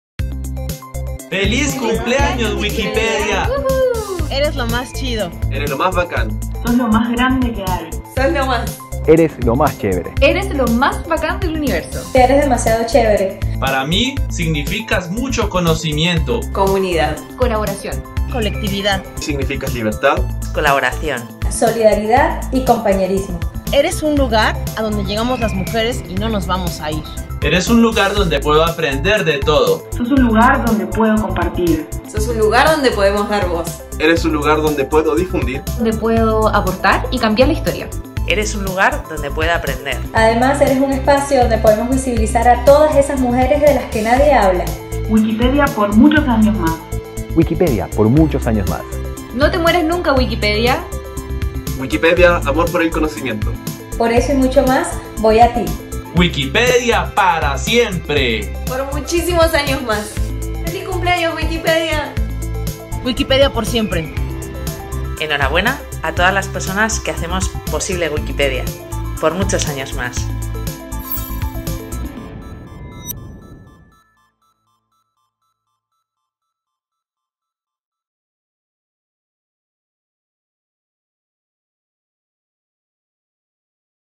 Wikipedia en español cumple 20 años
Wikipedia es bacán, chido, chévere y lo más!
Ayer fue su cumpleaños número 20Cañón de serpentina y te invitamos a escuchar lo que nuestra comunidad tiene para decir y que nos cuentes ¿qué es Wikipedia para vos?
¡Feliz cumple Wikipedia en español!